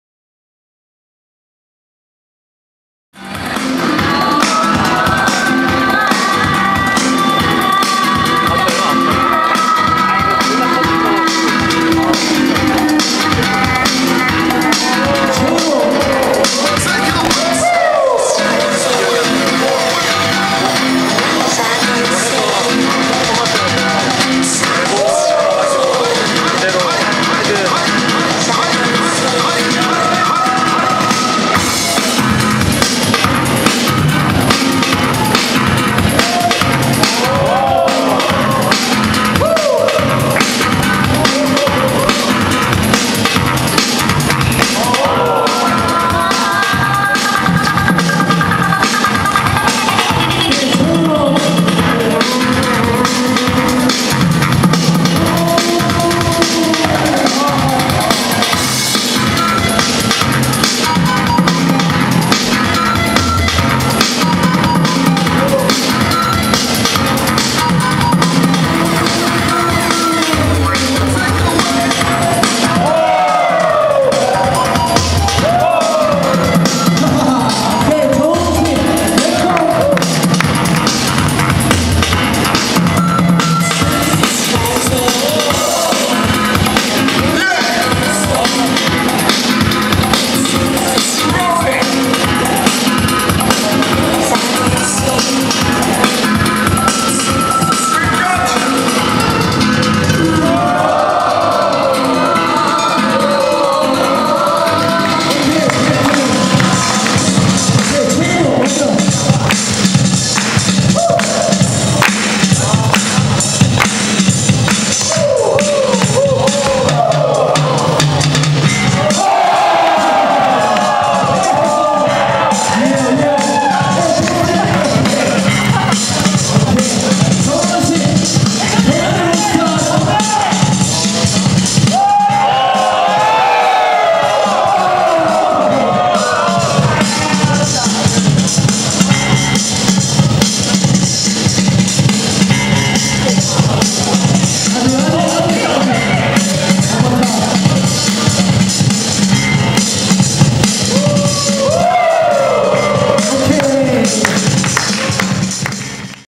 Skill Battle Final / 2UNSICK vs Tenbird / BREAK THE WALL / Allthatbreak.com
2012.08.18

BREAK THE WALL
- Jam with cypher for university bboys/bgirls

Skill Battle Final
2UNSICK (Baekseok Arts Univ.) vs Tenbird (Kyunghee Univ.)
win - 2UNSICK